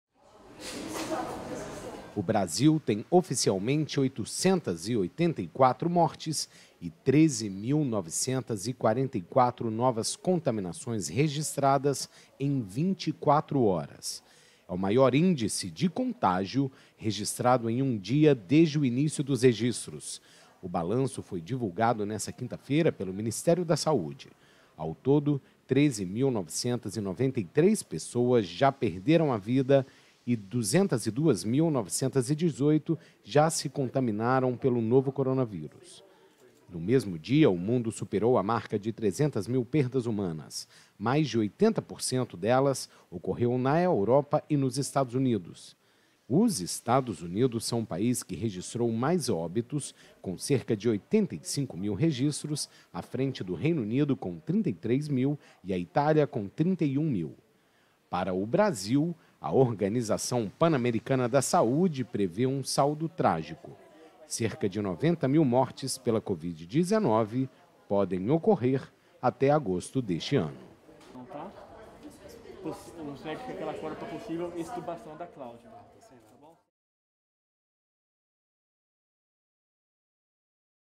Maior índice de contaminações | AFP
O Ministério da Saúde atualizou os números da Covid-19 no Brasil: em um dia, 884 pessoas perderam a vida pela doenças, enquanto 13.944 foram contaminadas - o maior índice registrado até o momento.